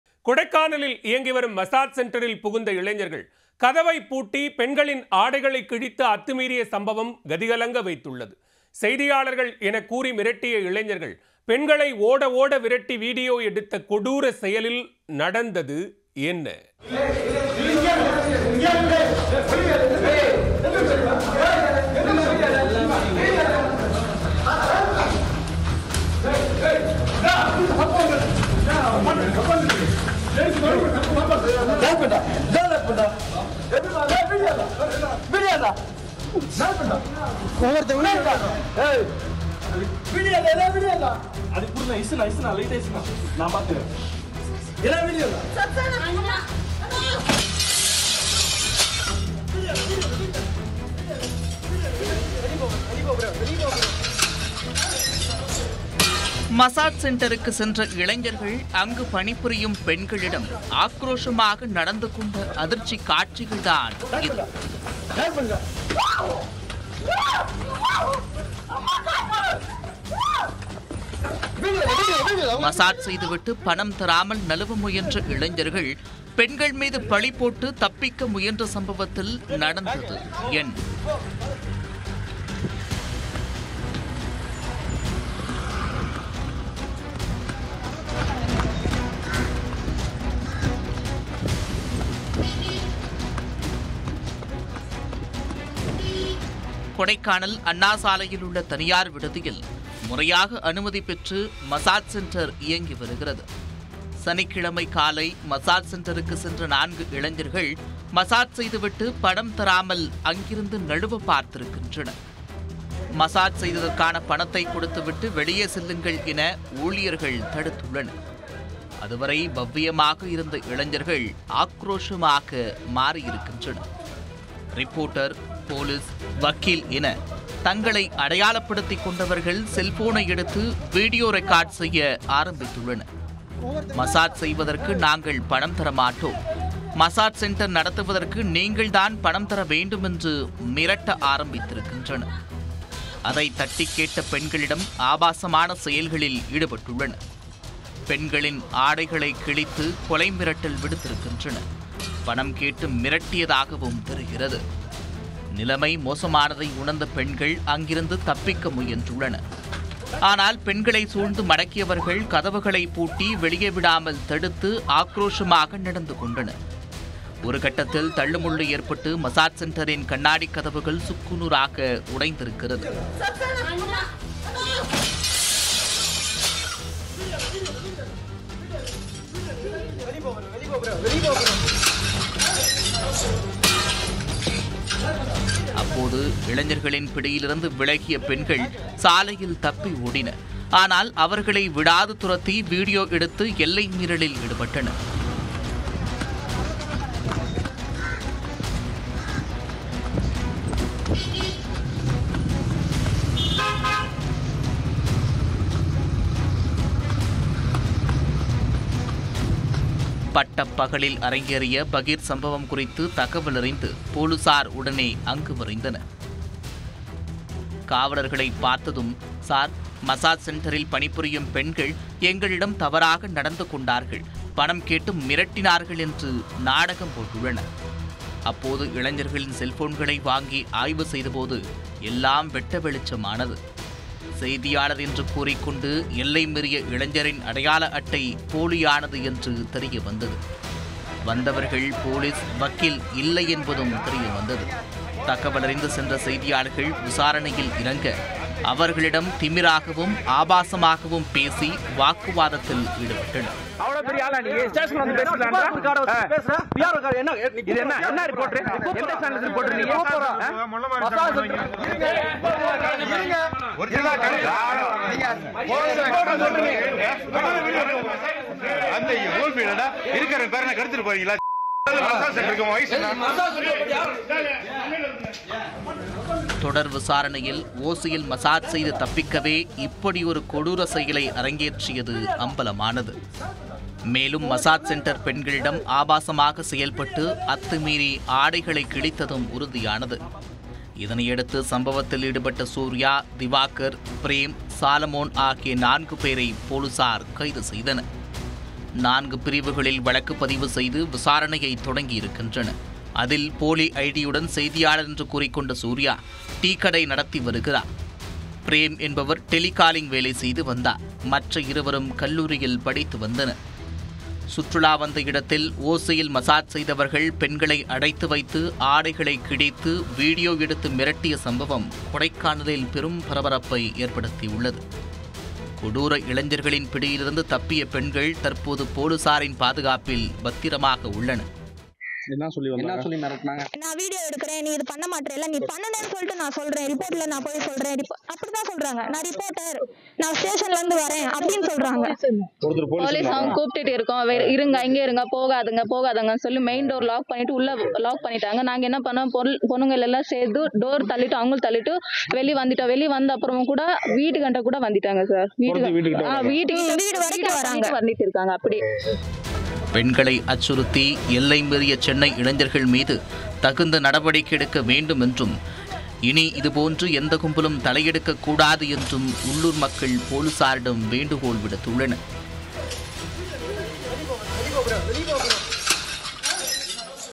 Crime Time | மசாஜ் செண்டரில் பகீர்! - பெண்களை பூட்டி வைத்து சித்ரவதை..
கொடைக்கானலில் இயங்கி வரும் மசாஜ் செண்டரில் புகுந்த இளைஞர்கள்,  கதவை பூட்டி பெண்களின் ஆடைகளை கிழித்து அத்துமீறிய சம்பவம் கதிகலங்க வைத்துள்ளது. செய்தியாளர்கள் எனக்கூறி மிரட்டிய இளைஞர்கள் பெண்களை ஓட ஓட விரட்டி வீடியோ எடுத்த கொடூர செயலில் நடந்தது என்ன?